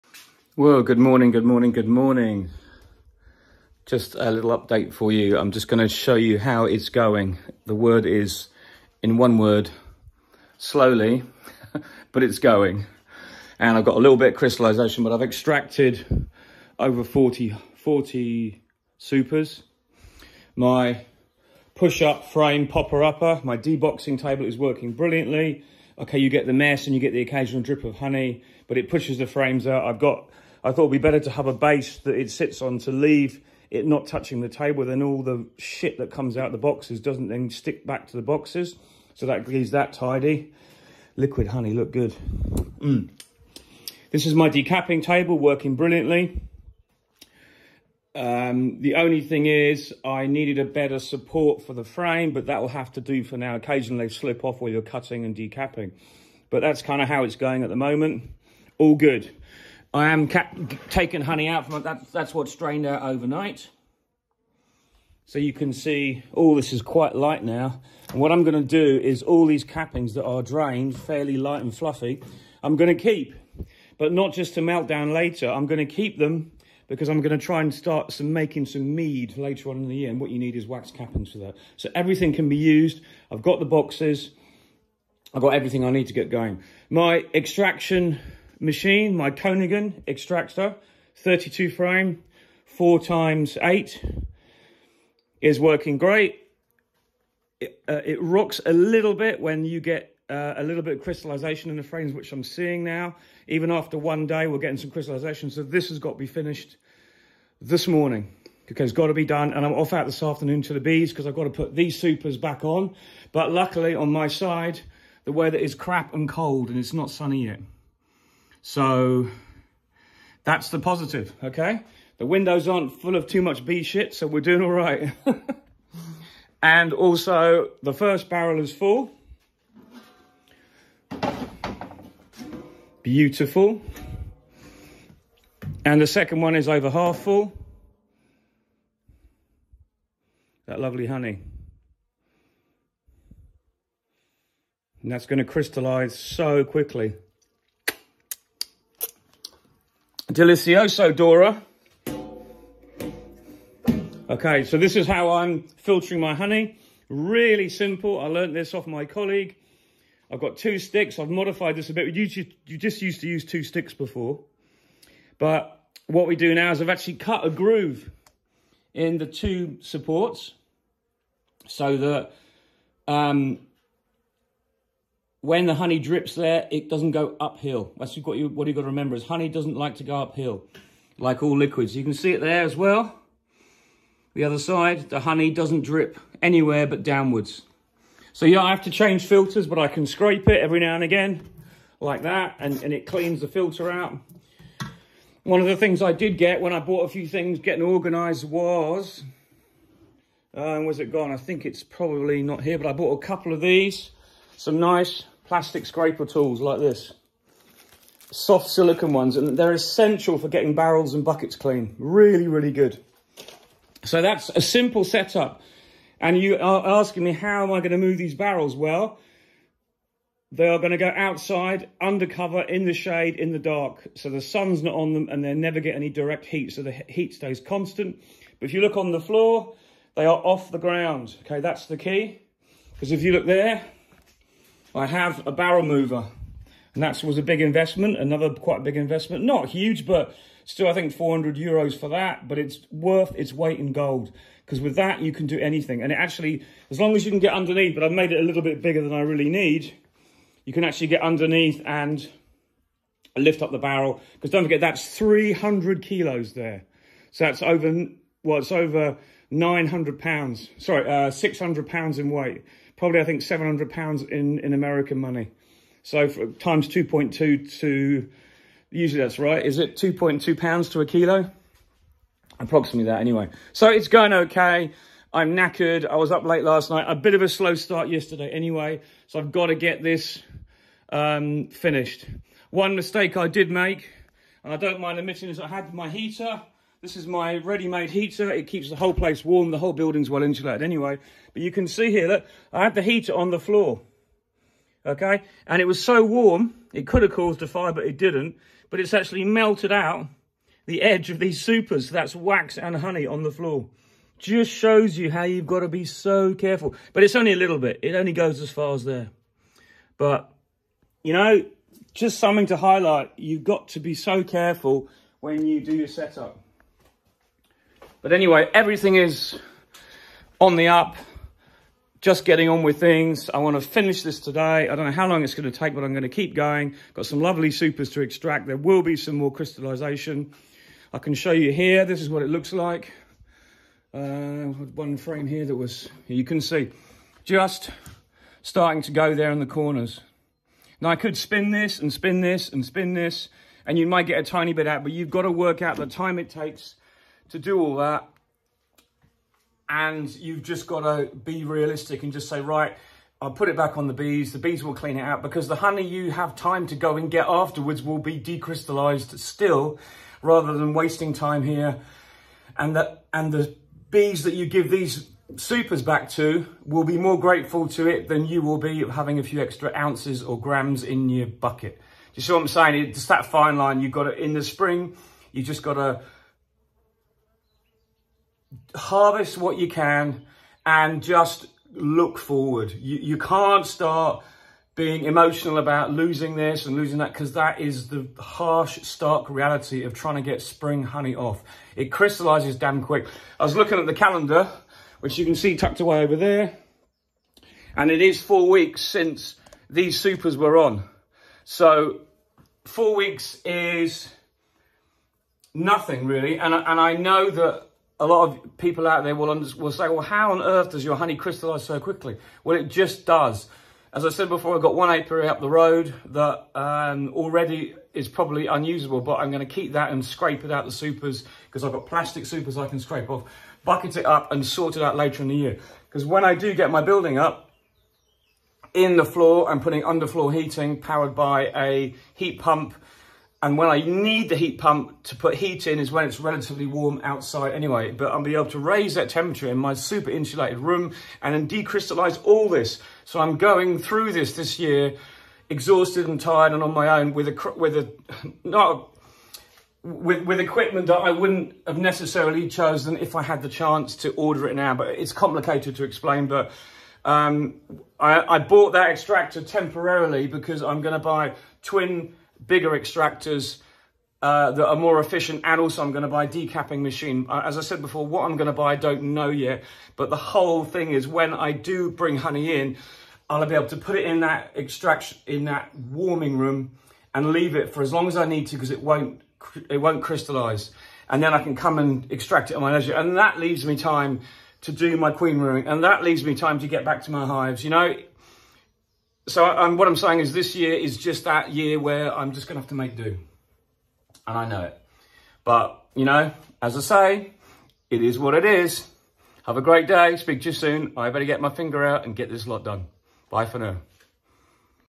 Hi Richard, how’s it going?
So,  an update on a walk round my temporary extraction room to show you how things are going when you’re extracting on your own. Some positives and negatives but overall we’re moving forward and getting most of the Honey off in a timely fashion.￼